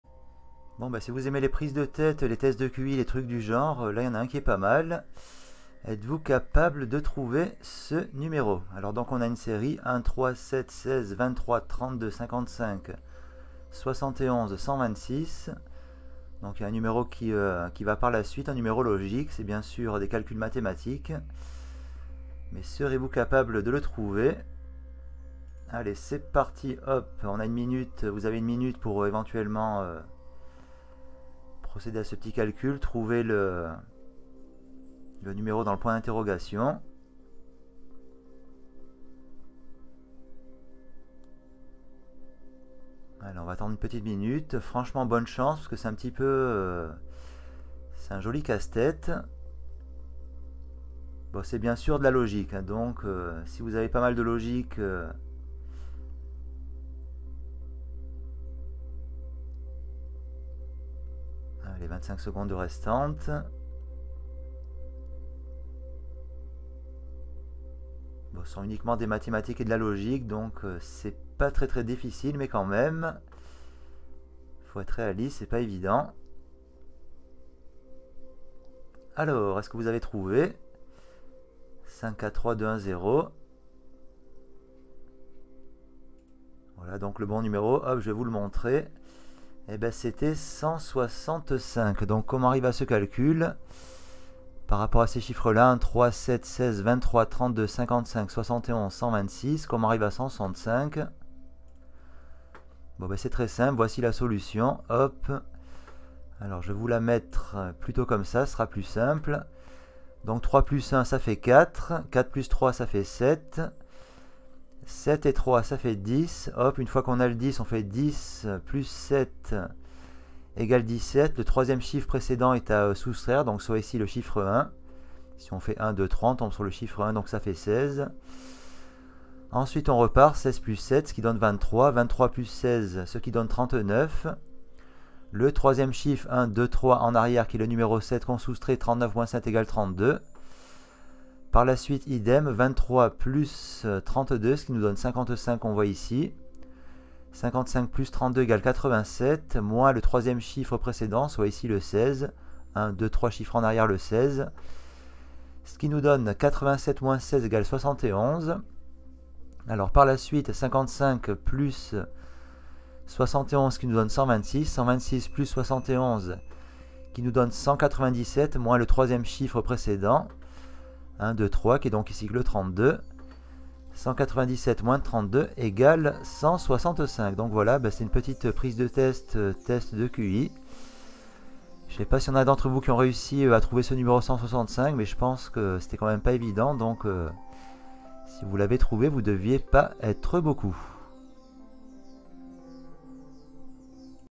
Test de Q.I. ❓ Test de logique mathématique 🔜 Trouver le numéro manquant
Serez vous capable de trouver le numéro manquant ? Ceci est de la logique mathématique ! Mais pas si facile que ça !